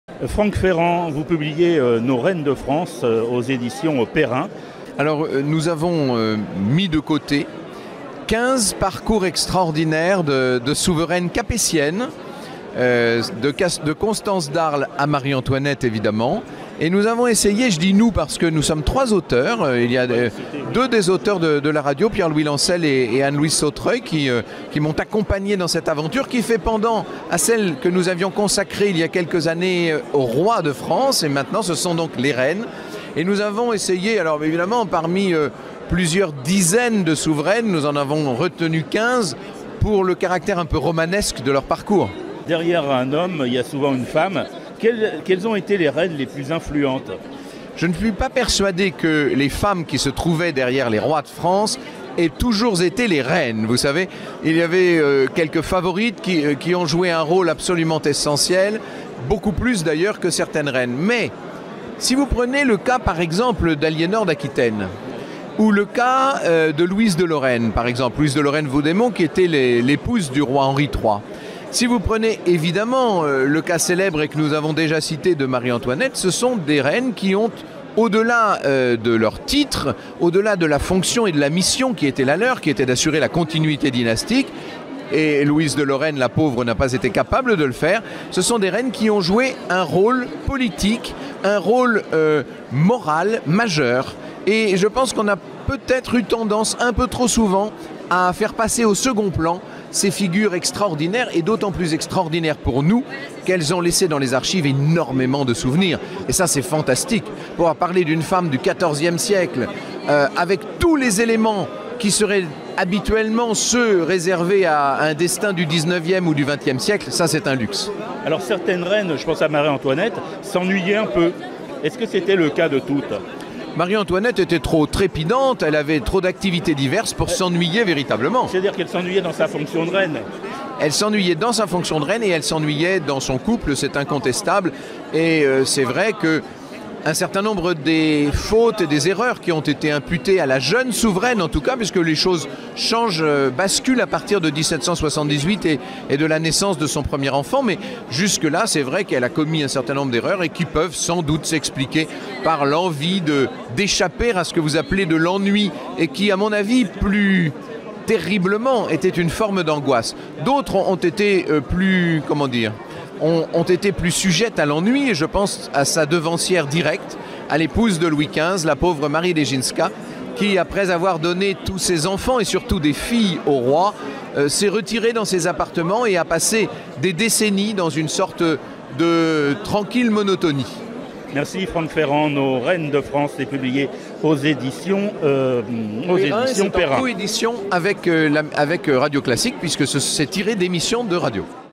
INTERVIEW DE FRANCK FERRAND "NOS"REINES DE FRANCE"
Les reines de France en majesté. Nées à l'étranger pour la plupart, souvent même issues de dynasties rivales, les princesses qui deviennent reines de France renoncent à une part d'elles-mêmes. Non par choix, non par sentiment – par devoir...